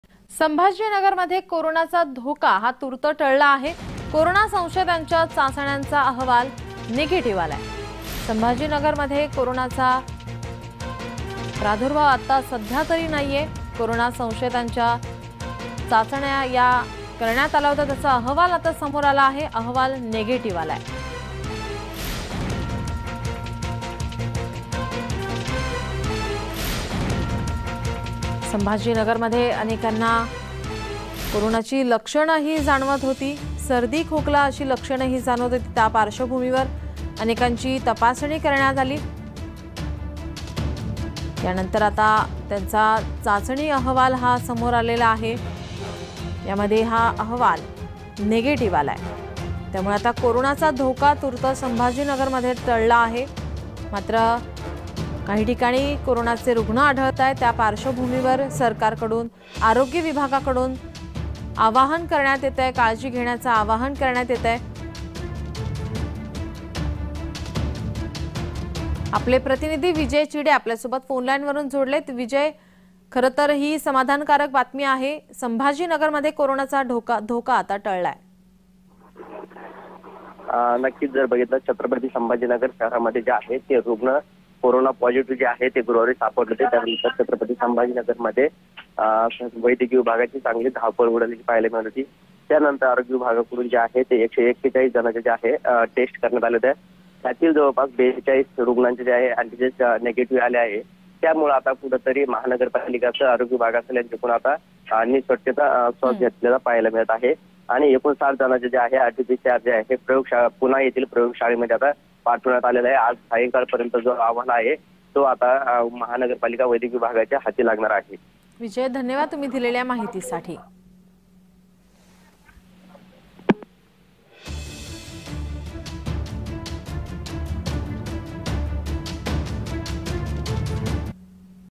Sambhaji Nagar | कोरोनाचा धोका तूर्त टळला | Marathi News
संभाजीनगरमध्ये कोरोनाचा धोका तूर्त टळला
कोरोना संशयितांच्या चाचण्यांचा अहवाल निगेटिव्ह